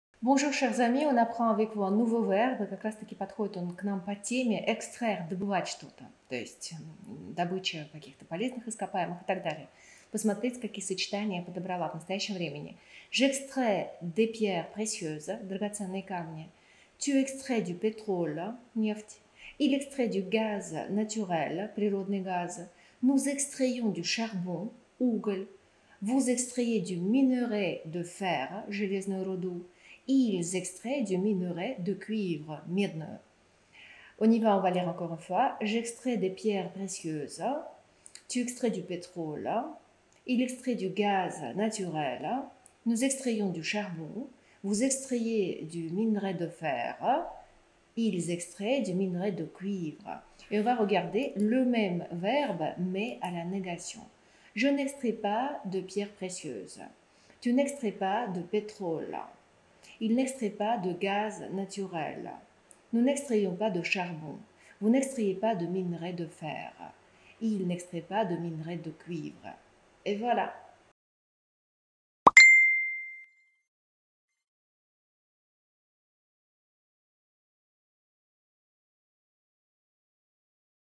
Cпряжение в présent №66: extraire (французский язык)
Сегодня знакомим вас с новым глаголом: extraire - добывать, извлекать.

В этом видео мы выучим спряжение глагола extraire в настоящем времени, а также несколько примеров употребления.